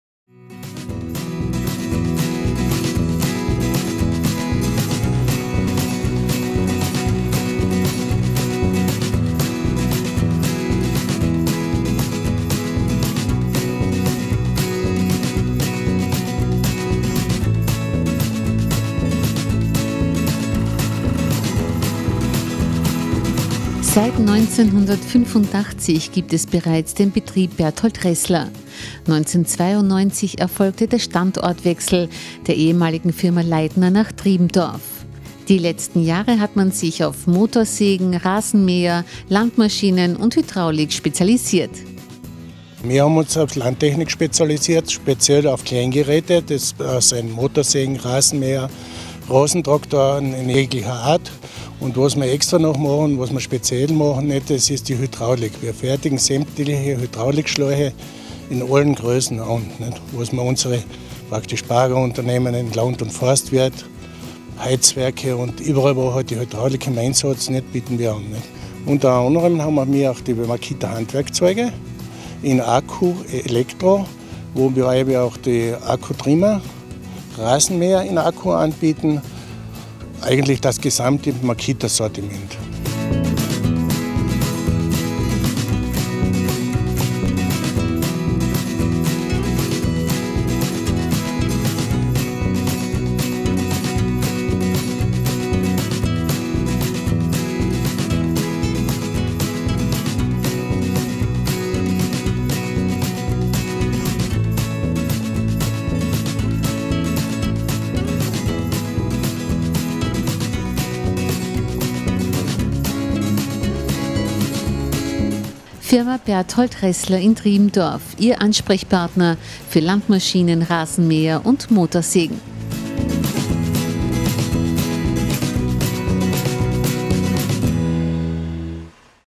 Berthold Reßler - Motorsägen / Rasenmäher / Landmaschinen
Fa. Berthold Reßler in Triebendorf ist ihr Ansprechpartner in Sachen Motorsägen, Rasenmäher, Lanmaschinen und Hydraulik.